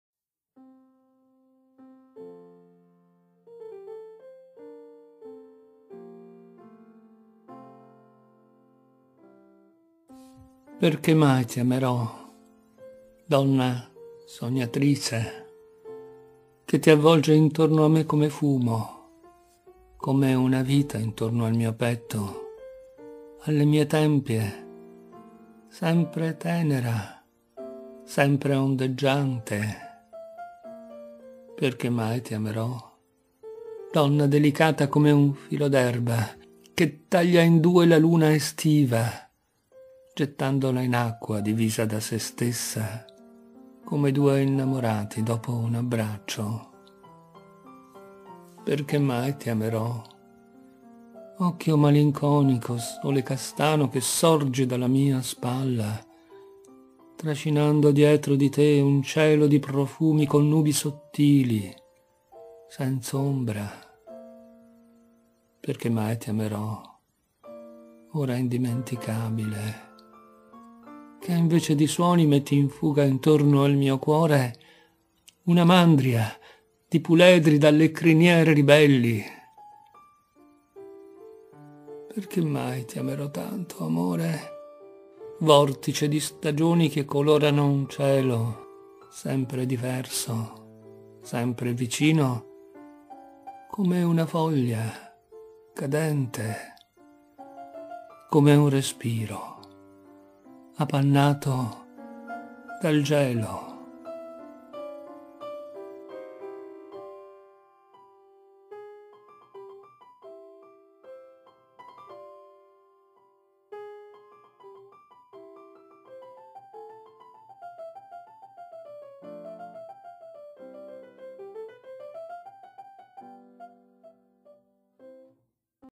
"perchè mai ti amerò" poesia di Nichita Stănescu
al piano valter zanardi